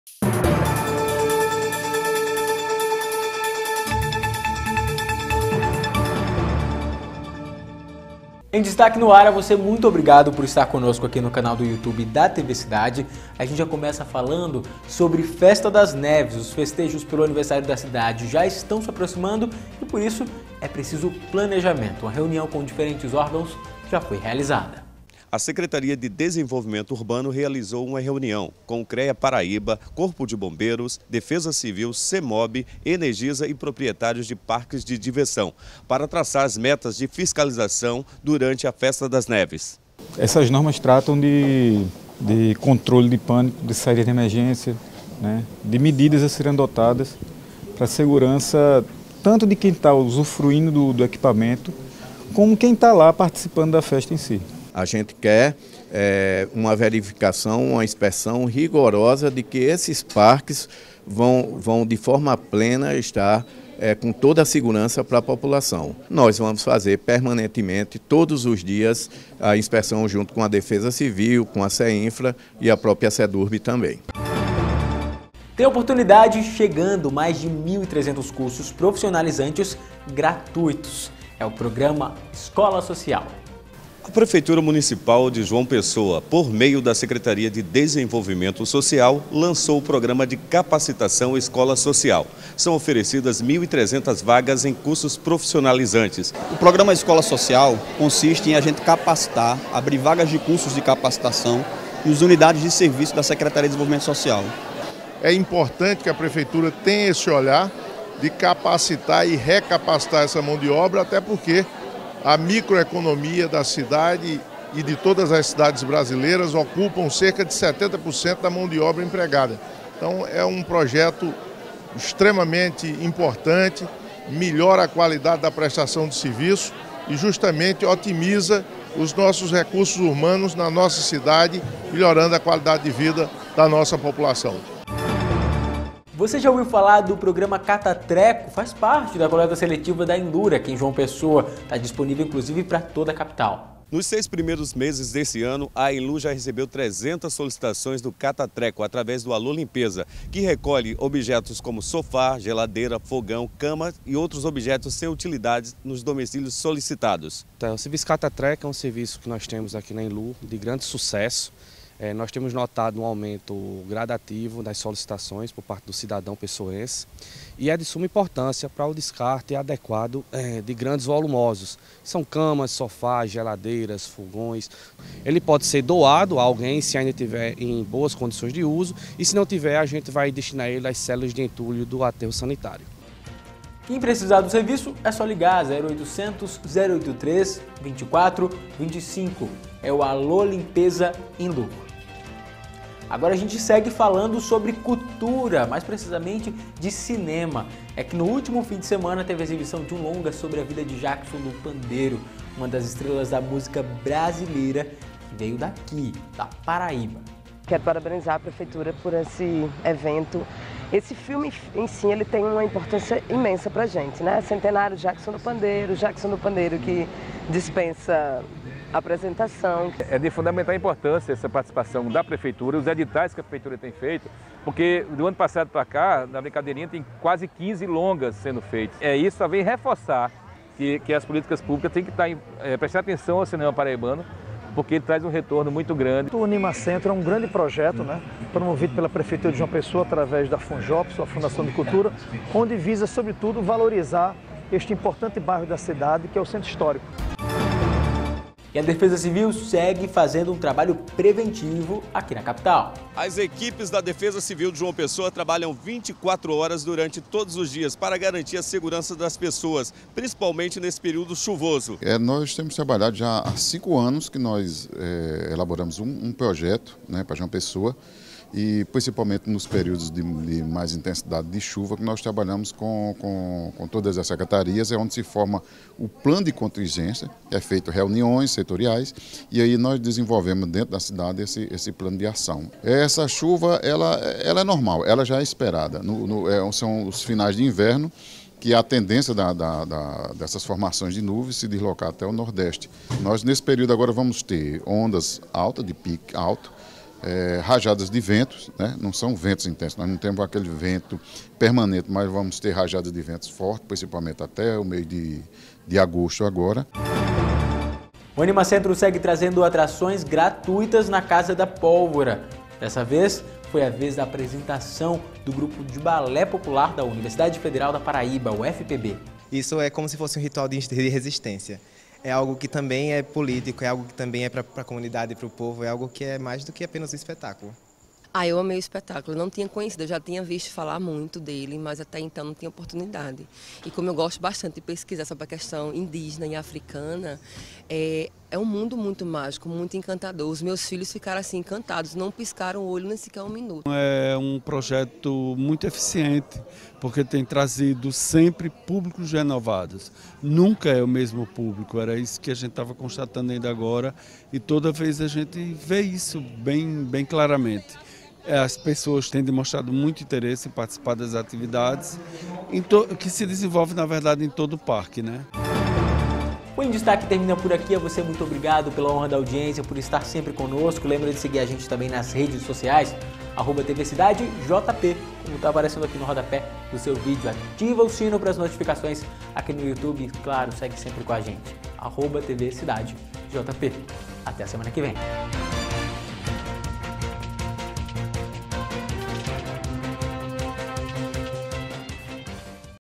EM DESTAQUE (22/07 à 26/07)
Quer ficar bem informado sobre as principais notícias que marcaram a semana em João Pessoa? Se liga no 'Em Destaque' que traz um resumo completo pra você. 😉